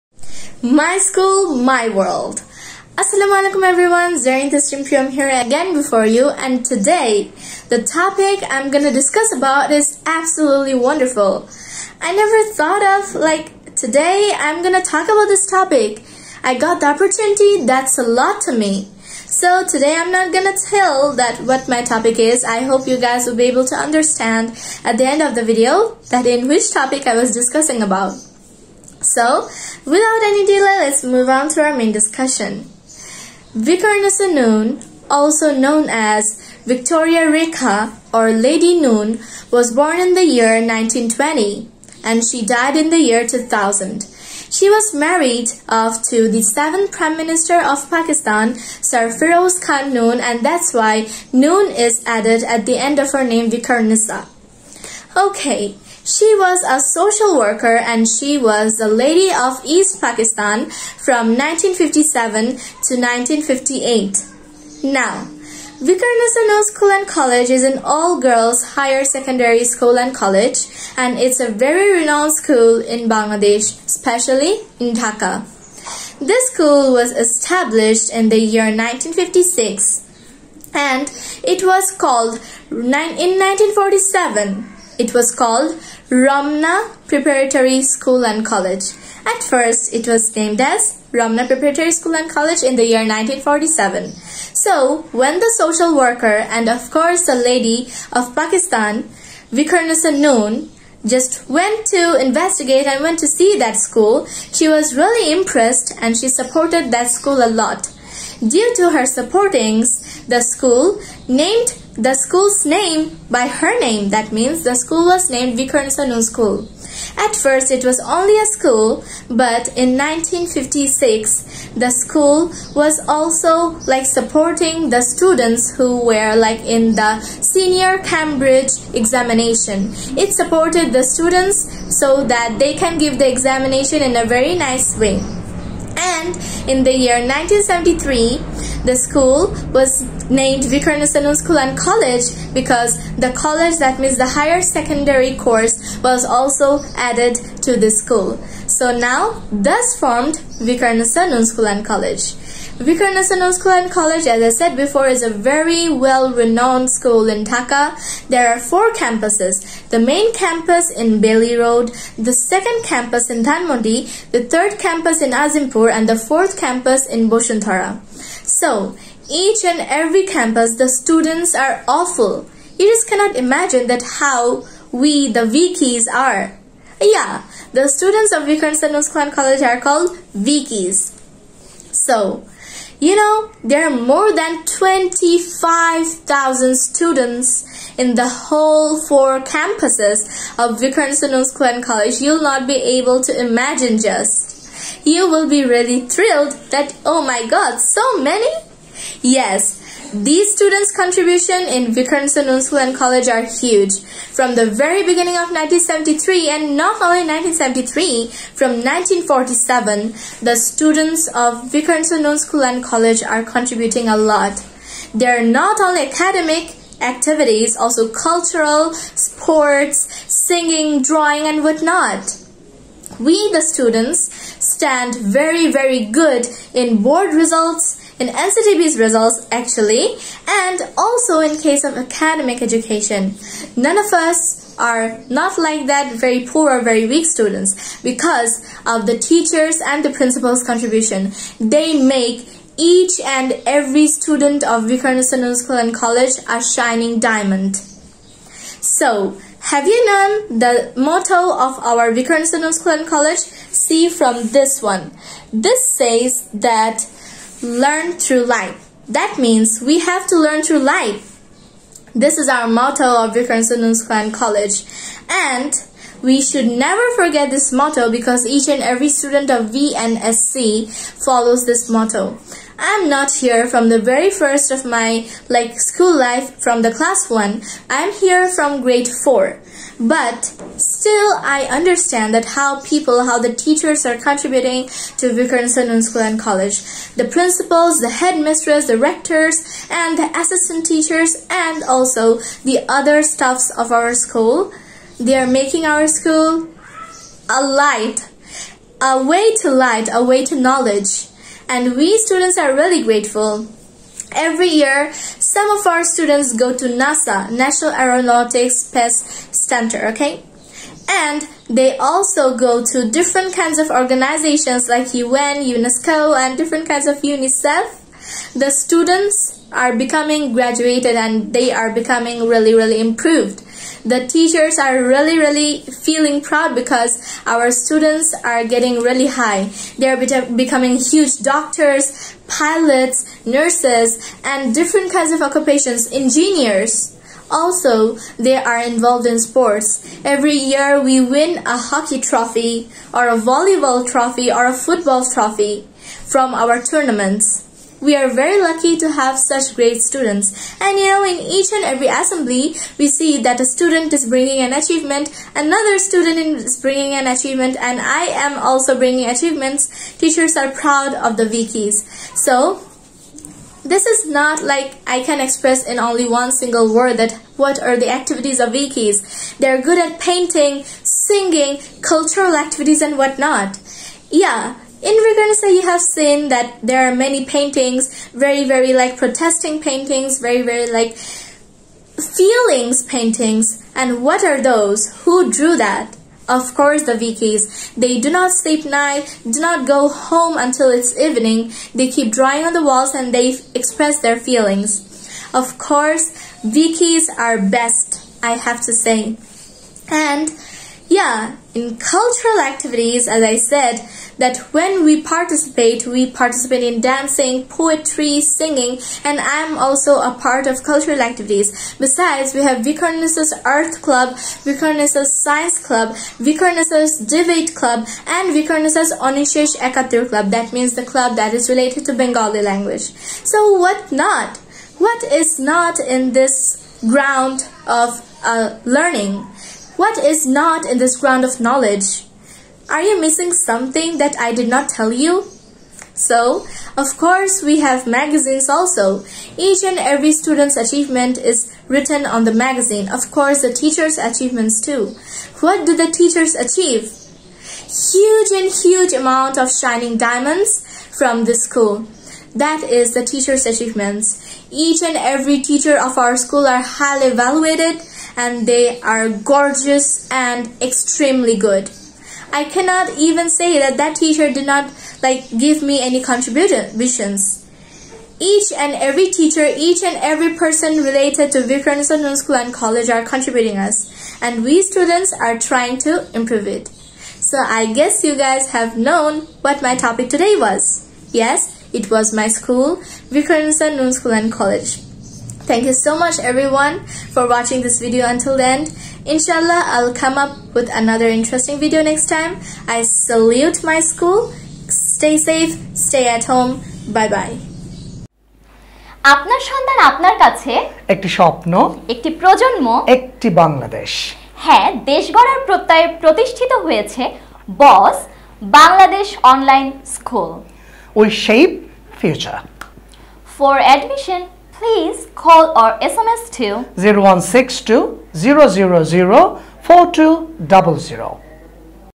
100 Extraordinary English Presentation-068 | By Zarin | Produced by TESOL BANGLADESH.
100 Extraordinary  English Presentation-068 | By Zarin | Produced by TESOL BANGLADESH.

Description :
Prepare to be amazed as Zarin delivers an exceptionally captivating English presentation! In this episode, witness the power of language as Zarin explores a fascinating topic with unparalleled eloquence and clarity. Gain valuable insights, improve your speaking skills, and be inspired by Zarin's extraordinary command of the English language.